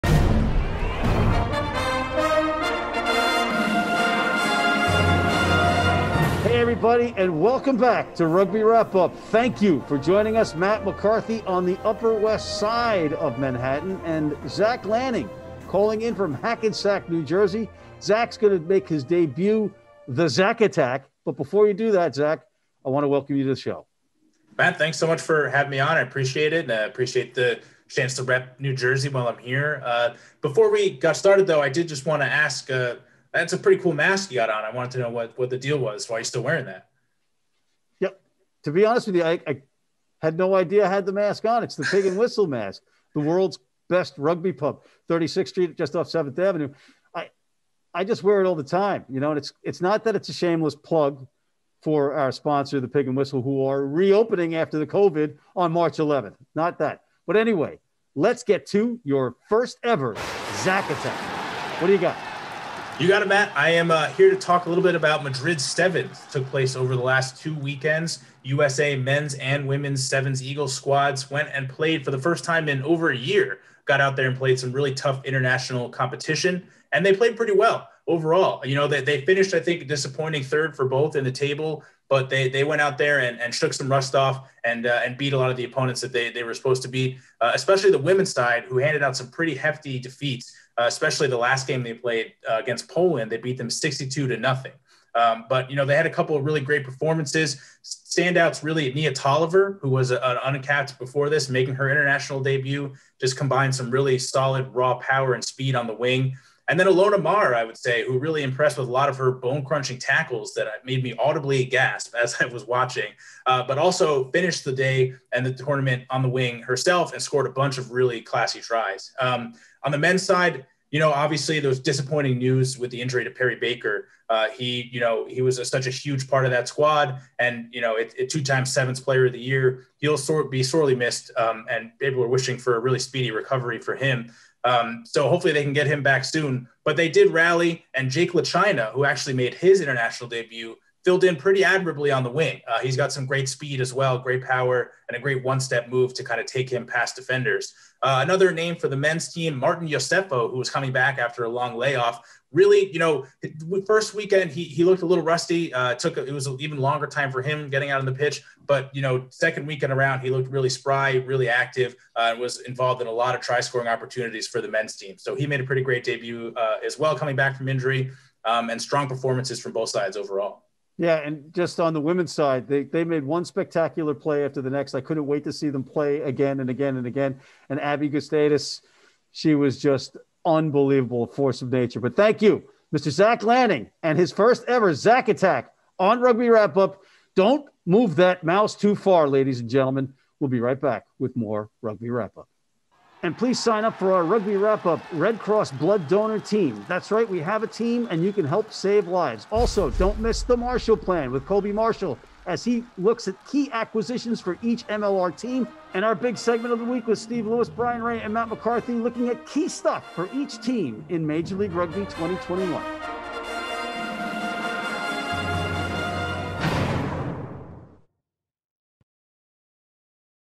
USA Rugby Women/Men 7s Review, Perry Baker Update | RUGBY WRAP UP
USA Rugby 7s &  Perry Baker Update | RUGBY WRAP UP

NEW YORK, NY – We’re back. With a bang. Or at least enough new content to keep you out of trouble for a bit… This week we have a whole show – 20+ mins including breaks – that you can watch or download at your leisure as a podcast. You can also watch/download the individual segments – because we know you’re busy.
-Major League Rugby 2021 Team Rundown with Steve Lewis, Bryan Ray & Matt McCarthy.
-The Marshall Plan: A fast new feature with Colby Marshall re key MLR off-season moves.
-The Zach Attack: Highs and Lows for the USA Rugby 7s Women & Men

Watch... Listen/Download the Podcast version... Please share and join our weekly newsletter

-Twitter @RugbyWrapUp, @Matt_McCarthy00, @JonnyLewisFilms, @Junoir Blaber, @JWB_RWU, @Luke Bienstock, @Ronan Nelson, @MeetTheMatts, @Declan Yeats.